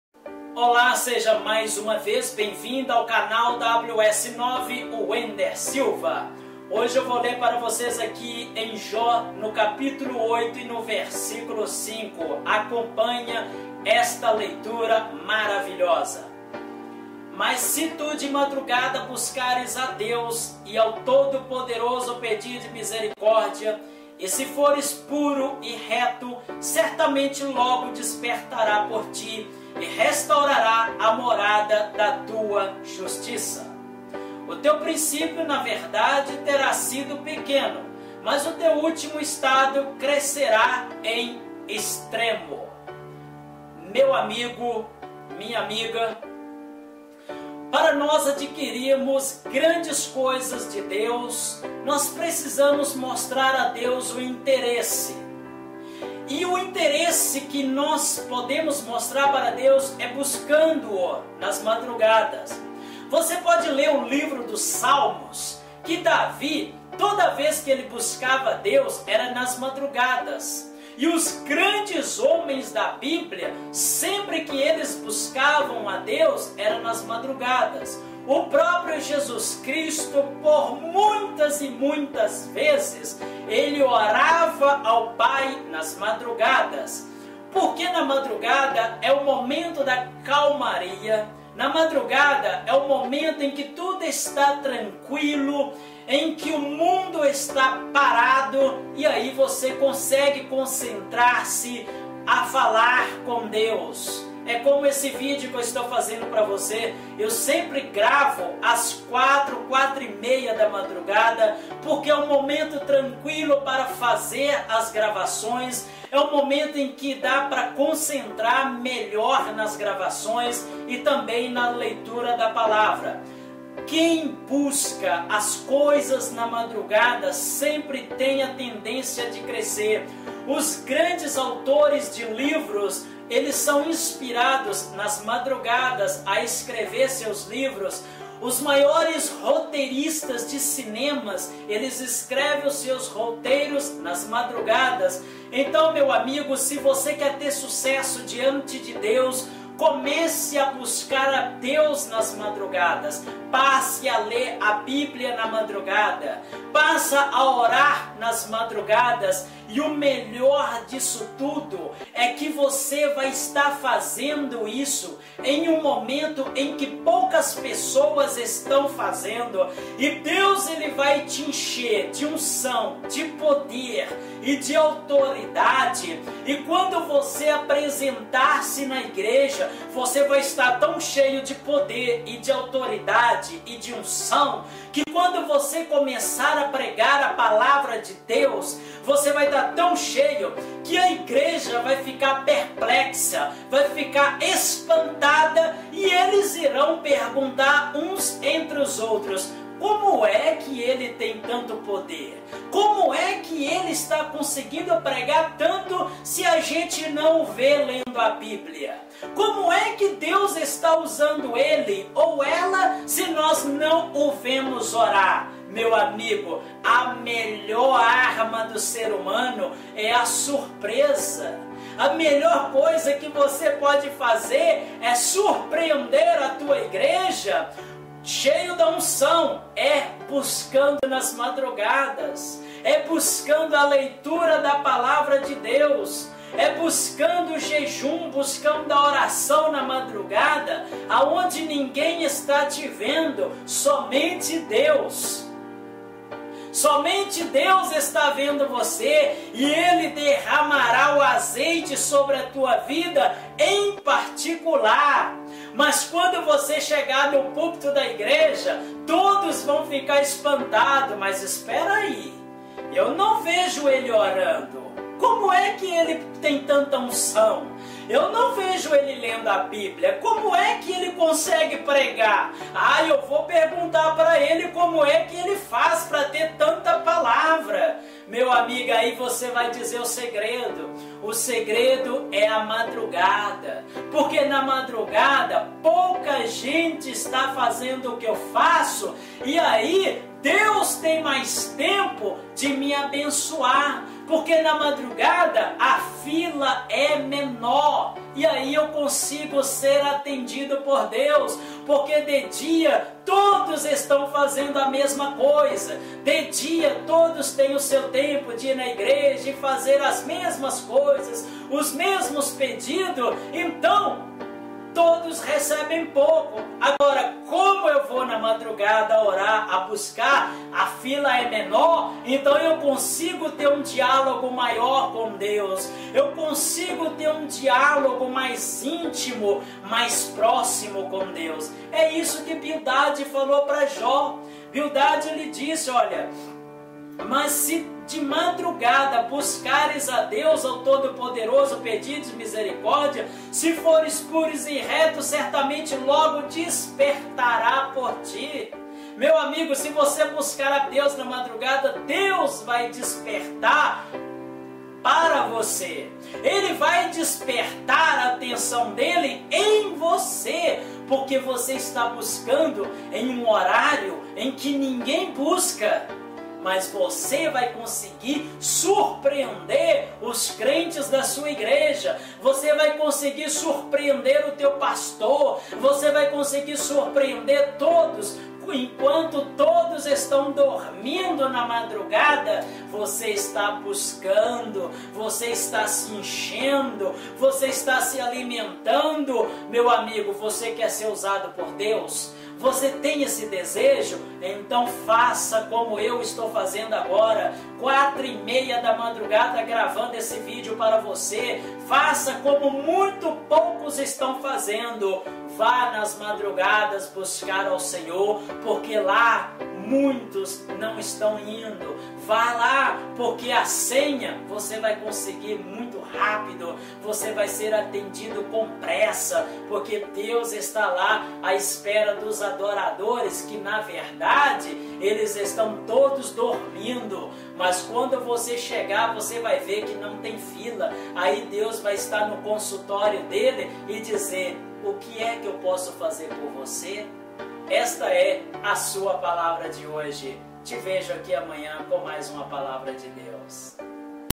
SAIBA AQUI A MELHOR HORA DE FALAR COM DEUS
Veja aqui o melhor momento e a melhor hora de falar  com  DEUS ,  se você quer ver  a resposta de  DEUS  em sua vida então veja a hora certa de falar com DEUS ,  e nunca mais você  vai  ficar sem resposta,